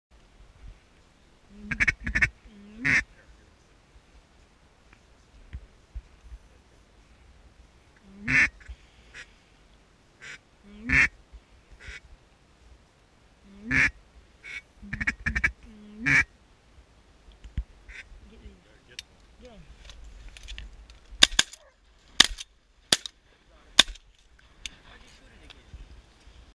Single Mallard Drake
A pair of mallards coming into the decoys at Sauvie Island WMA in Oregon. Successful harvest of 1 mature mallard drake.